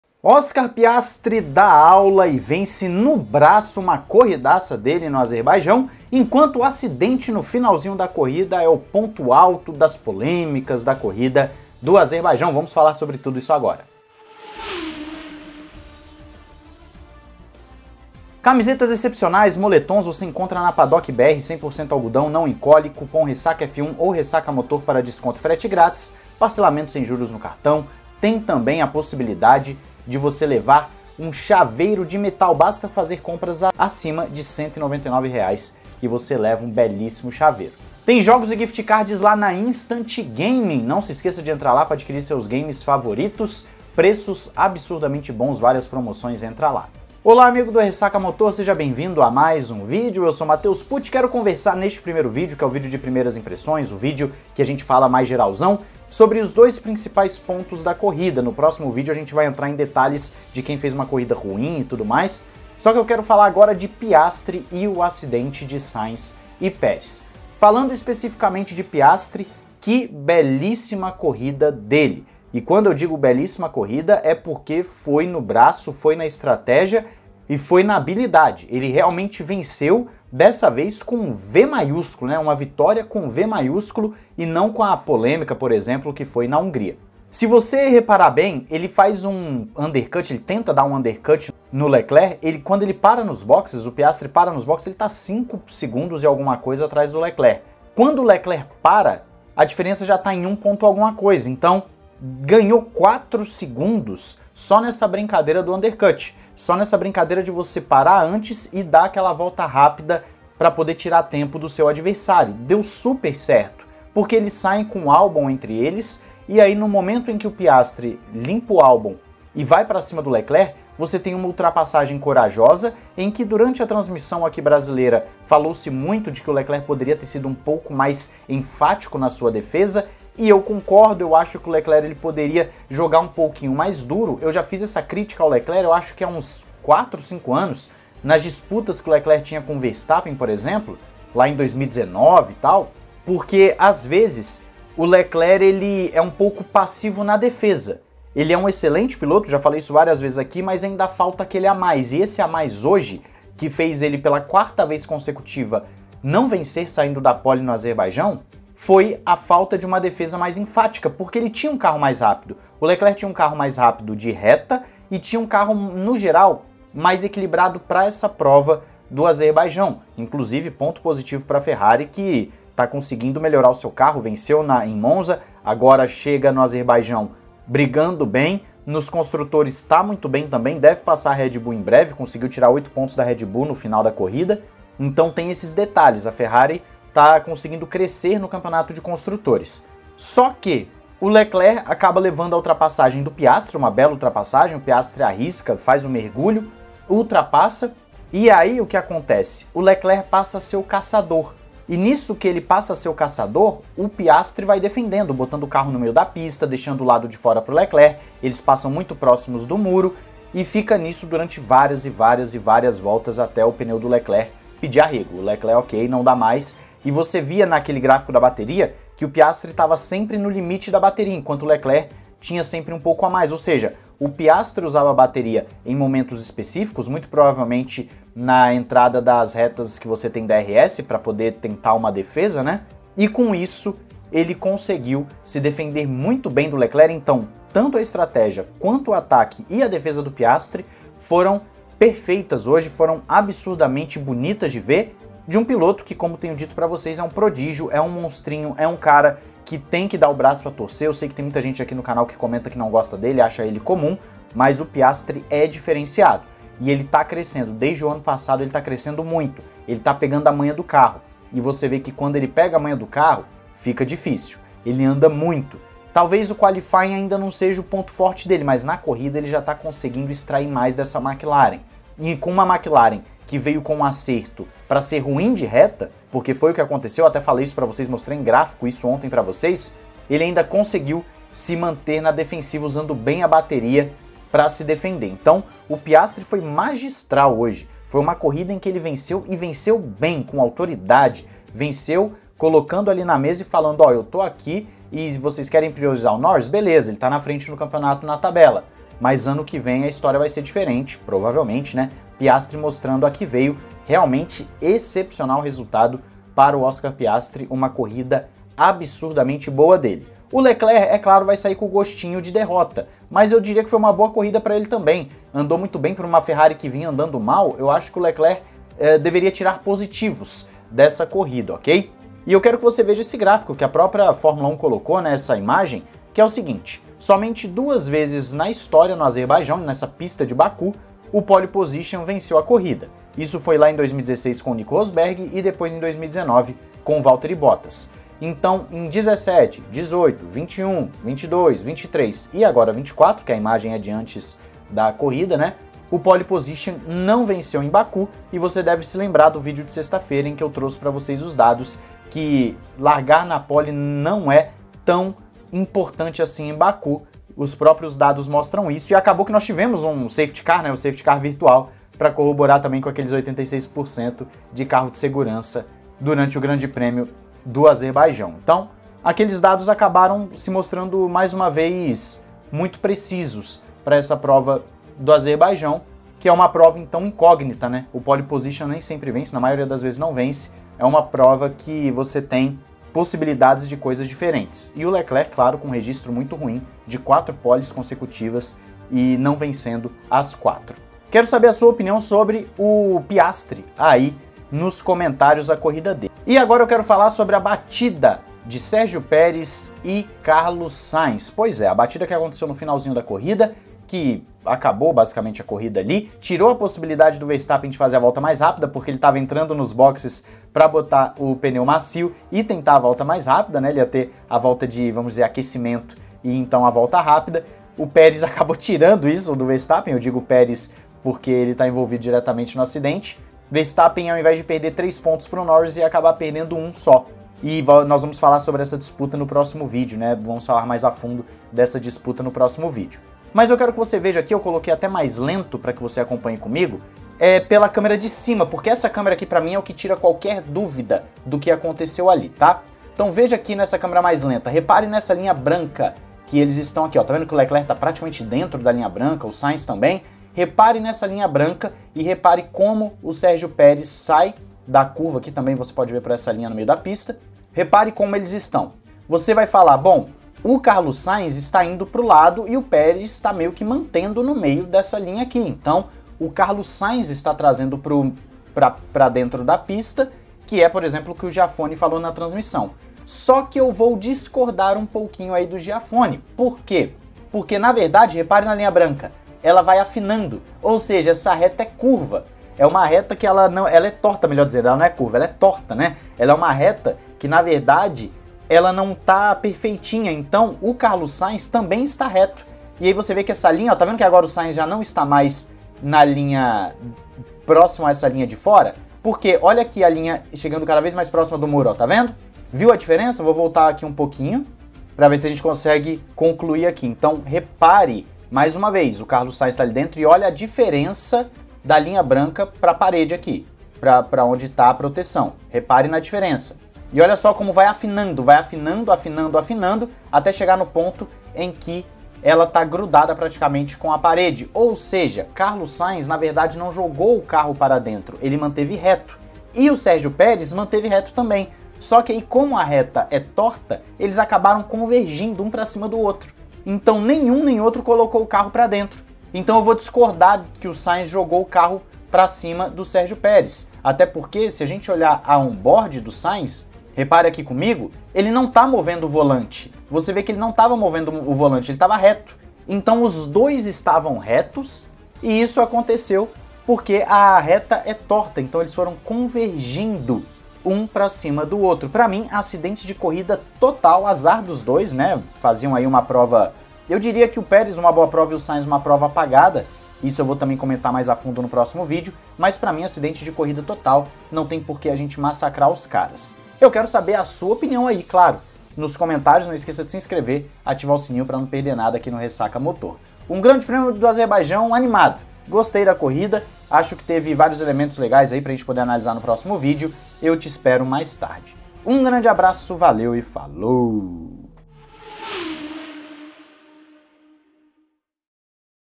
PIASTRI DÁ SHOW E TEVE BATIDA FEIA ENTRE SAINZ E PEREZ (TEM CULPADO?) GP AZERBAIJÃO
👍 PARCEIROS

Sergio Milani
@scuderiamilani (Twitter)

📰 *Fontes:*

João 3:16

00:00 - Introdução
00:47 - Aula de Oscar Piastri
05:22 - Pole x vitória em Baku
06:36 - Sainz x Perez, quem é culpado?
09:05 - Onboard de Sainz